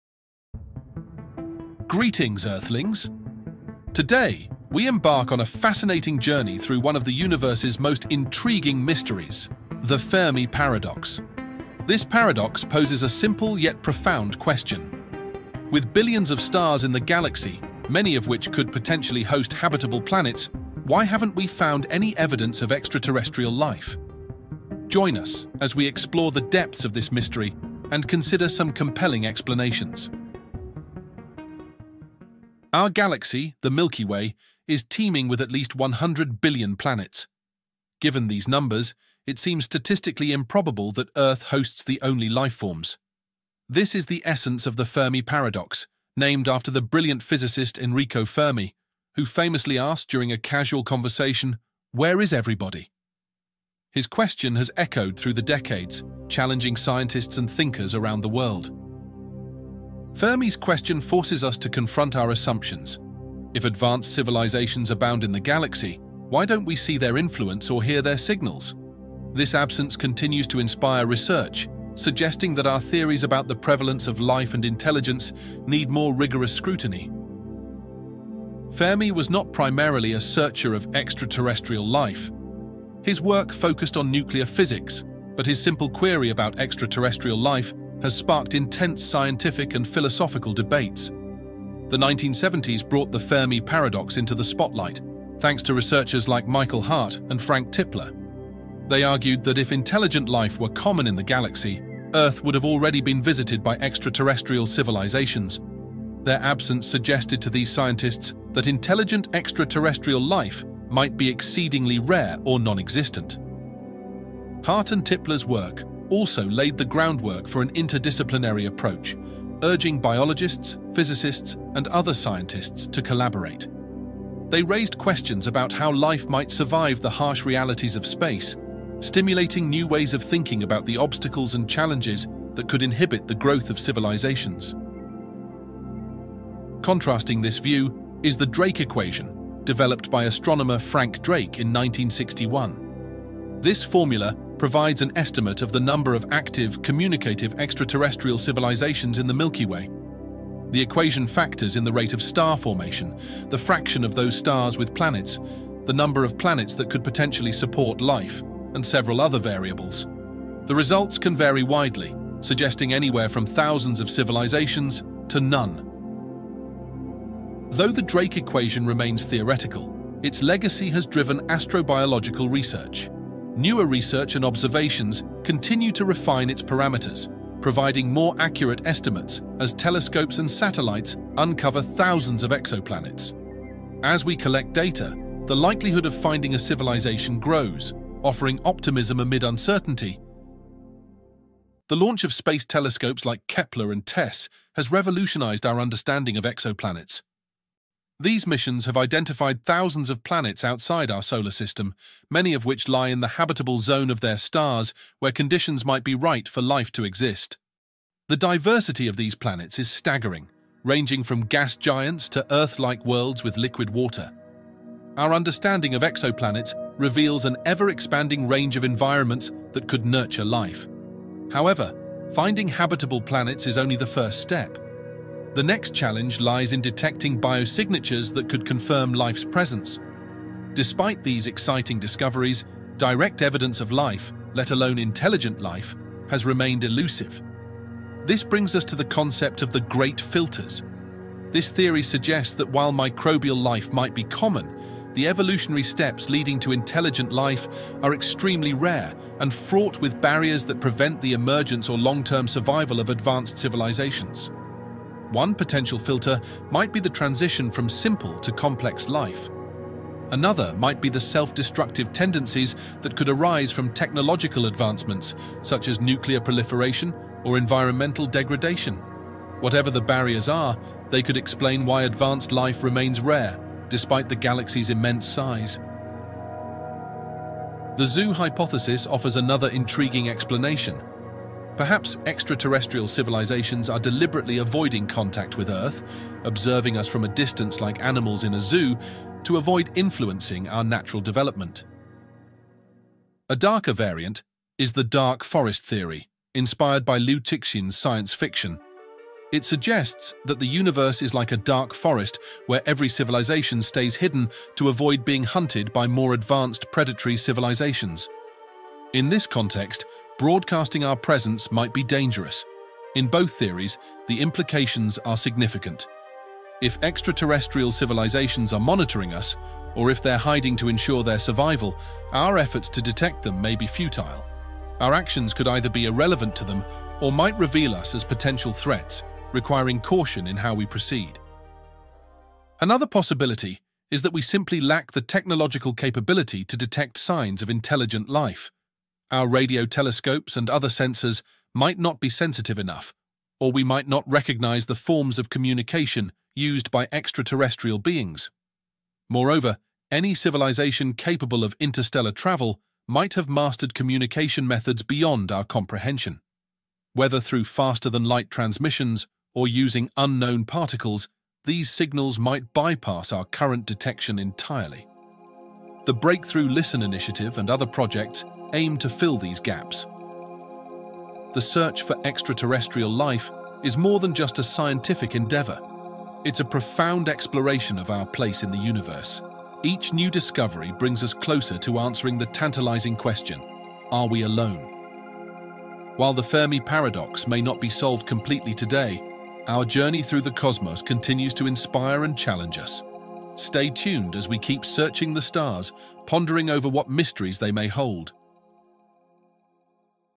Are We Alone In The Universe: The Fermi Paradox
The Fermi Paradox highlights the contradiction between the high likelihood of extraterrestrial civilizations existing in the vast, star-filled universe and the complete lack of evidence or observed contact with such civilizations. Named after physicist Enrico Fermi, who famously asked, "Where is everybody?" the paradox challenges scientists to explain why, amid billions of potentially habitable planets, we have yet to detect any signs of alien life.